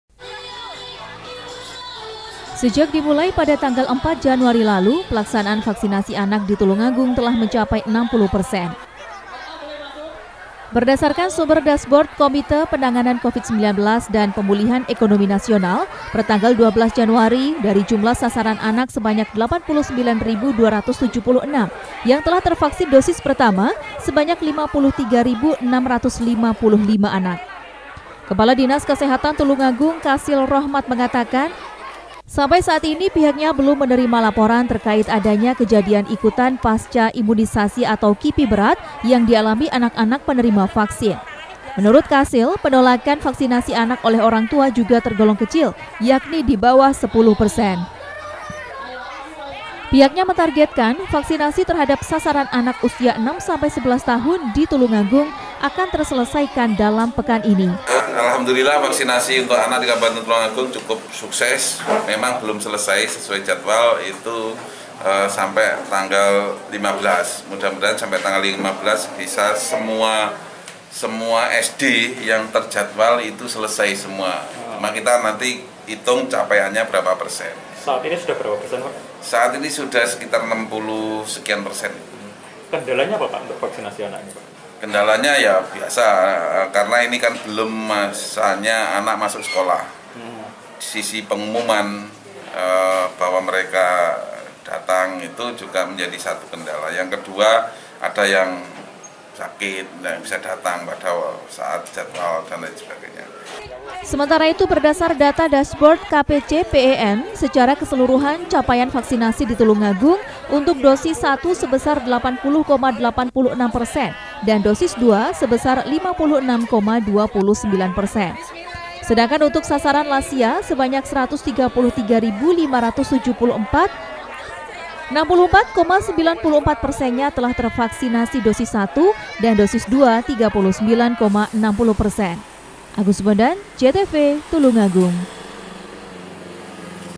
TULUNGAGUNG - 53 Ribu Anak Telah Disuntik Vaksin Dosis Pertama
Sebanyak 53 ribu lebih anak usia 6 hingga 11 tahun di tulungagung telah menerima vaksinasi covid-19. Jumlah tersebut merupakan 60 persen dari total sasaran sebanyak 89 ribu anak . Proses vaksinasi ditargetkan akan selesai dalam pekan ini.